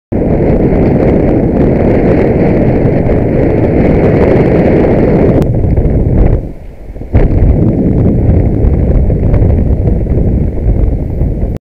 RLJ - scared me
Come on, give me a chance! I hit about 1/4 of a second of the start of the amber but the car sat at red clearly decided it was his turn and nearly broadsided me.